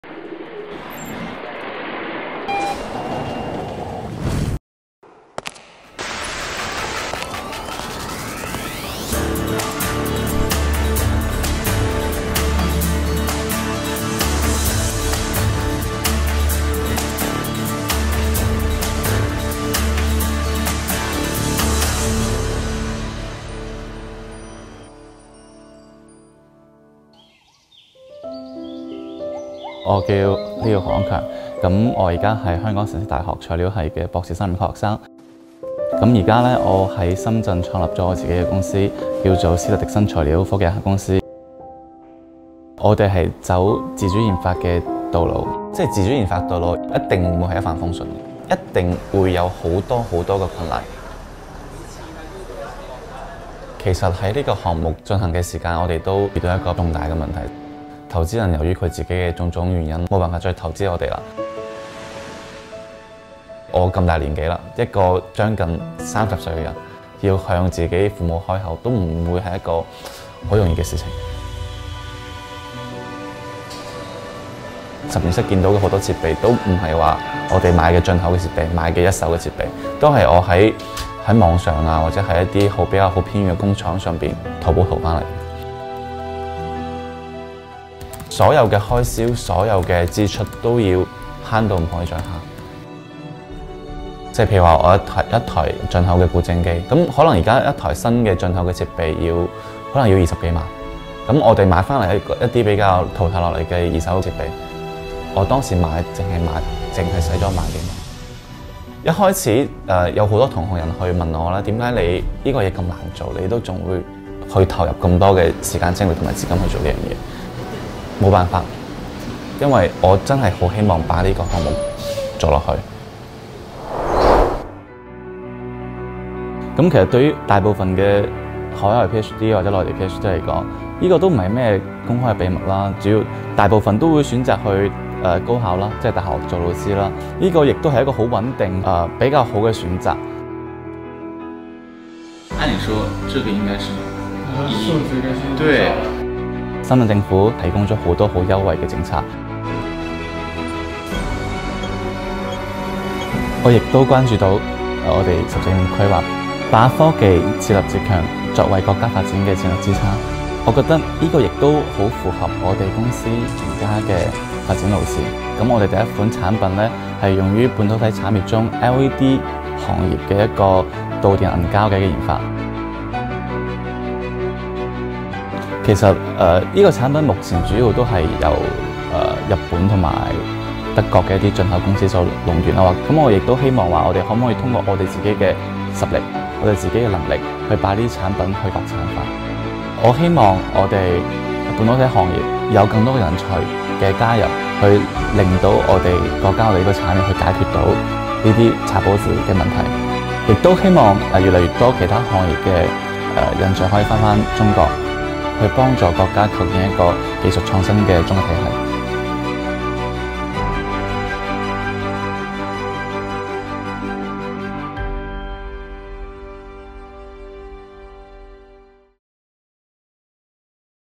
My homeland and I: Moving forward together
Leo Ho is a postdoctoral fellow in the Department of Materials Science and Engineering at City University of Hong Kong. He went to Shenzhen to establish his own company. He said his company's current development path, which has made self-reliance and self-improvement in science and technology a strategic pillar, is in line with China's 14th Five-Year Plan.